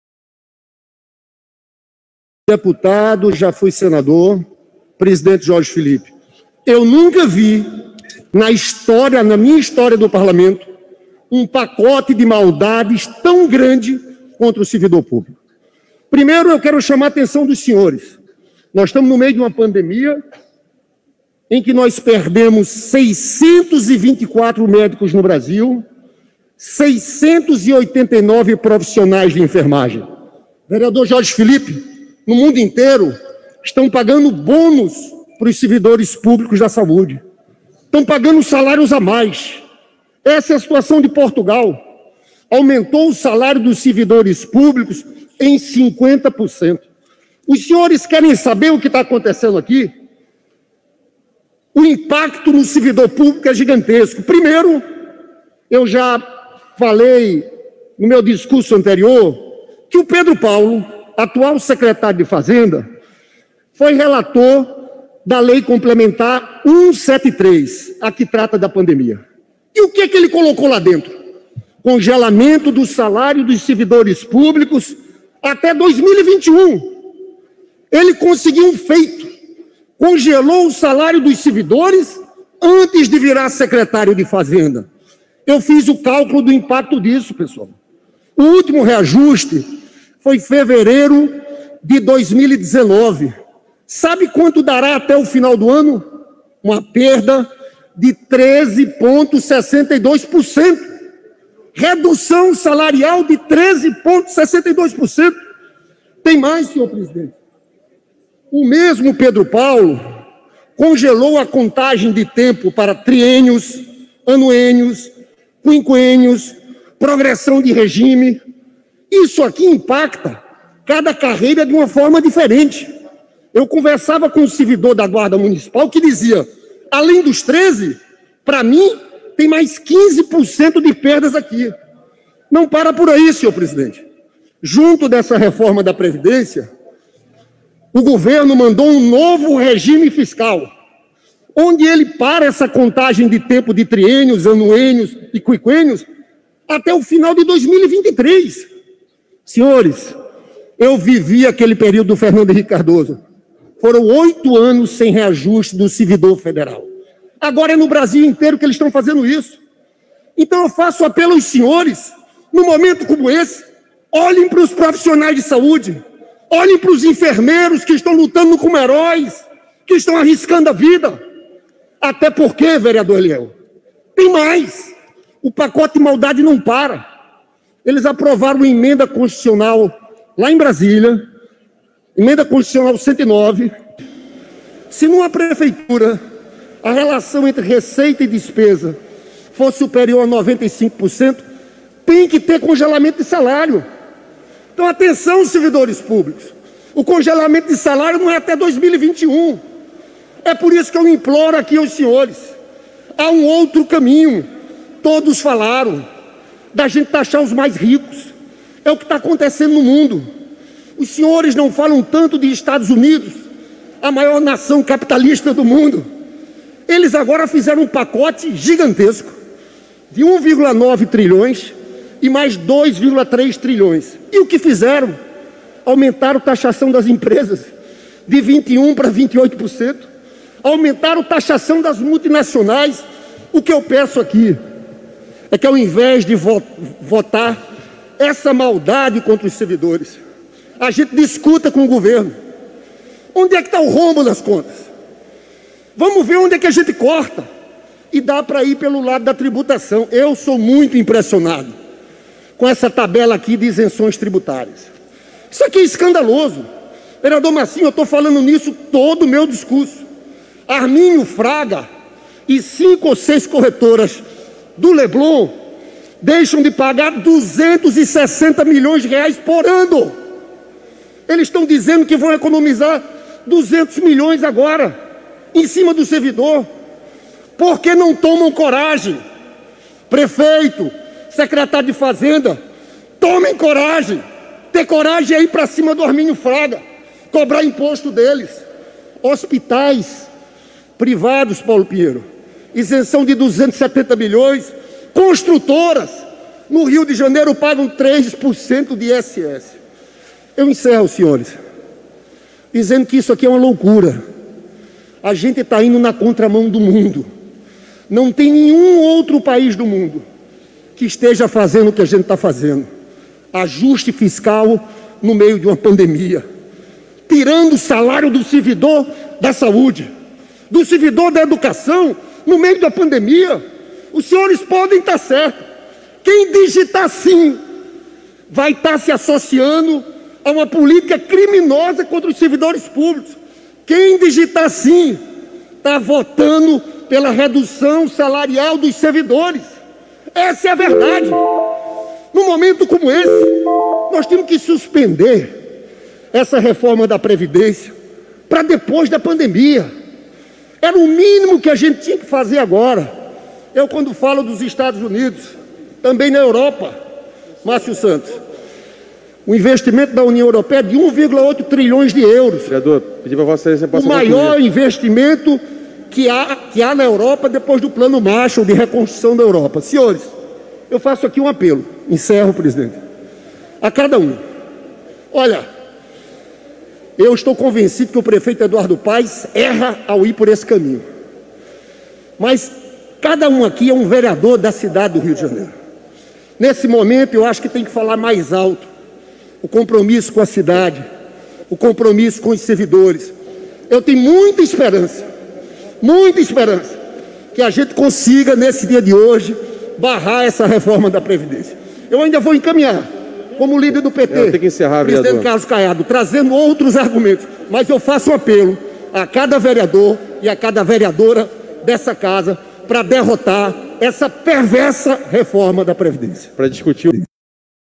O servidor não pode pagar essa conta!
Na contramão de outras medidas adotadas no mundo, a prefeitura do Rio insiste em retirar direitos dos servidores municipais. A Reforma da Previdência dos servidores não deveria ser discutida no meio de uma pandemia em que milhares de servidores da saúde perderam suas vidas. O servidor não pode pagar essa conta!